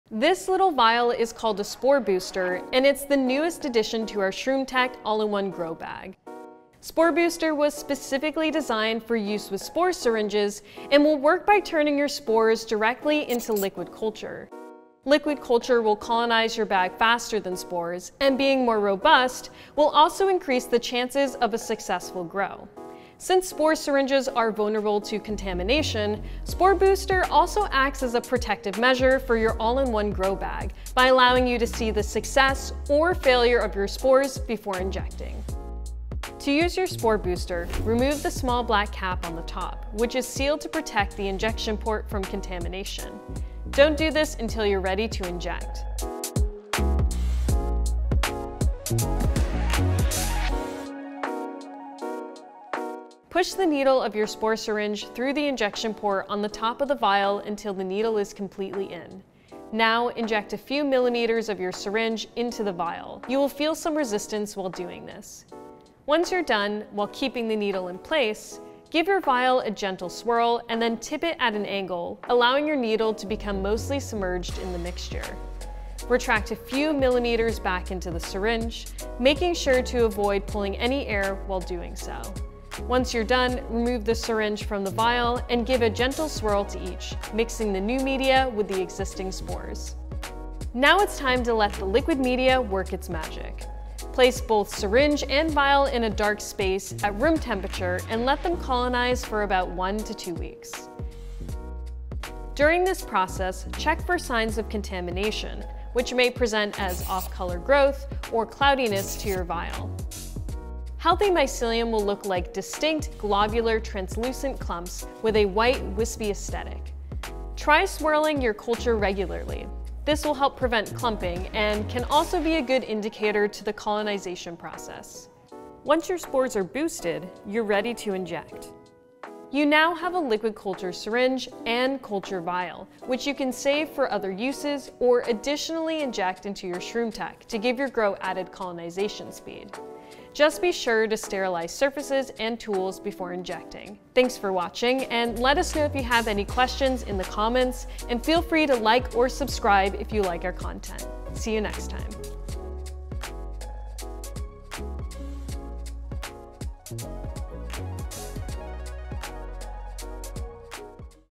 How to use Spore Boostr with Shroomtek | The NEWEST Mushroom Growing Hack | QUICK GUIDE
Spore Boostr is the newest addition to the ShroomTek family, and it's designed to to increase the speed of your grow by germinating spores in your syringe, creating a faster growing "liquid culture". Spore Boostr also allows you to catch contamination before you inject!

Learn how to use it and achieve your best mushroom grow in this video!

Chapters:

00:00 - What is a Spore Boostr? Introduction.
00:36 - How to use a Spore Boostr with a spore syringe
01:45 - Identifying contamination
02:06 - Injecting your ShroomTek